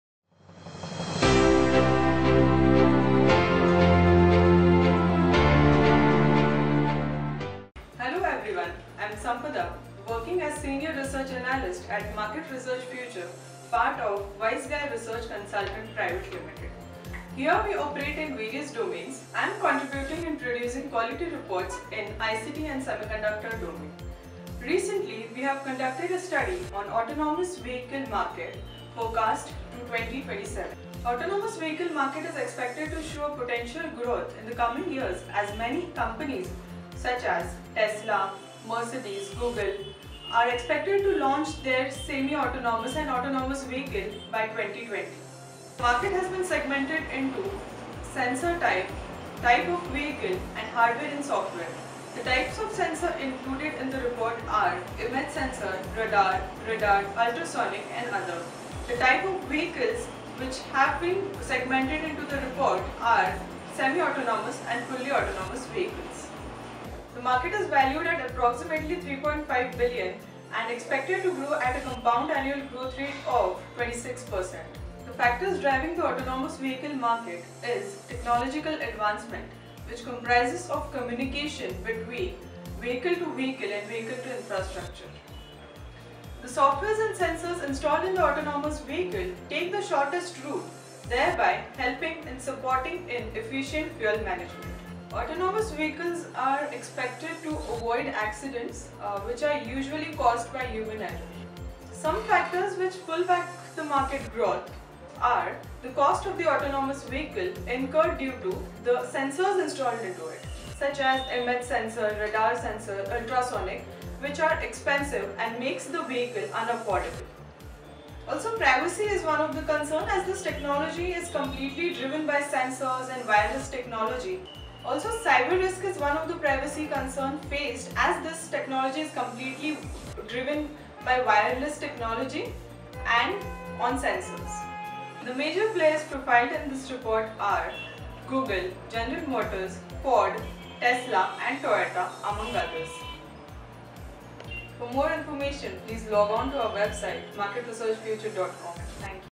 Autonomous Vehicles Market expected to boost as Tesla, Mercedes & Google are investing in millions
Marketresearchfuture.com predicts the autonomous vehicle market to reach around USD 65 billion in 2027 from USD 3.5 billion in 2015, to grow at a CAGR of ~26% between 2016 and 2027.

The development in autonomous vehicle industry by major players including Tesla, Mercedes, Google, and Toyota with advance features such as cruise control and collision avoidance is expected to boost the growth in coming years of autonomous vehicles market.

The major contributors are expected to launch their own versions of semi-autonomous and fully autonomous cars by the year 2020.
The Autonomous Vehicles market has tremendous opportunities in the upcoming years as companies are investing millions of dollars in the R&D and for the development of autonomous vehicle. In the same context, a French Company Induct Technology has developed the first commercial autonomous vehicle which is on the roads of France and Switzerland. Driverless cars sense the environment and their sensor guides the control system about the objects in all the directions. It is expected that once the autonomous vehicles hit the road; it will boost the market immensely.

Moreover, Google’s autonomous cars are in testing phase and are been running on roads in California. The driverless cars are completely driven by the software and sensors to take shortest route by monitoring traffic. Also, autonomous vehicles address pedestrians and potential obstacles thereby offering facility to control cars by drivers and passengers whenever needed. These features are expected to show significant growth for the autonomous vehicle industry.

In our market research report, the autonomous vehicle market is segmented by sensor type, hardware and software, type of vehicle, and region. The sensor type comprises of ultrasonic, radar, lidar, image sensor, among others. The vehicle type includes Semi-Autonomous vehicles and Fully Autonomous vehicles.  Regional and country level autonomous vehicle market analysis was conducted to derive at a market number. With historical data, secondary & primary research, MRFR analysts came to the conclusion that North America region accounted for the largest market share of global autonomous vehicle market in 2015. North America is followed by Europe and APAC in the growth of autonomous vehicle market.